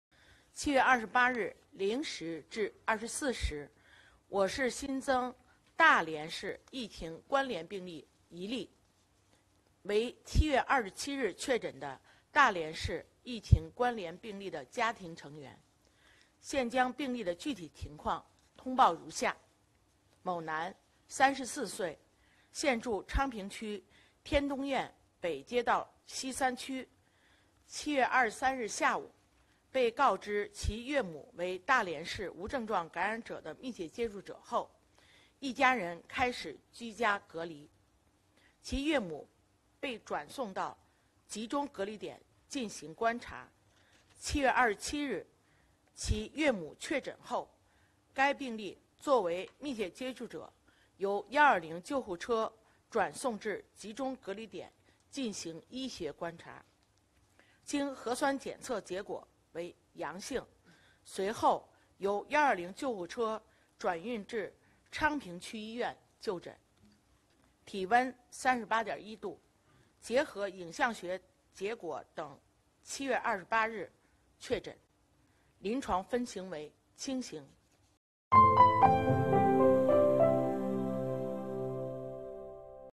北京：新增1例新冠肺炎病例系大连关联病例女婿
7月29日，北京市疾控中心副主任庞星火介绍，7月28日0时至24时，北京市新增大连市疫情关联病例1例，为7月27日确诊的大连市疫情关联病例的女婿。
该病例具体情况通报如下：某男，34岁，现住昌平区天通苑北街道西三区。7月23日下午，被告知其岳母为大连市无症状感染者的密切接触者后，一家人开始居家隔离，其岳母被转送到集中隔离点进行观察；7月27日，其岳母确诊后，该病例作为密切接触者由120救护车转送至集中隔离点进行医学观察，经核酸检测结果为阳性，随后由120救护车转运至昌平区医院就诊，体温38.1℃，结合影像学结果等7月28日确诊，临床分型为轻型。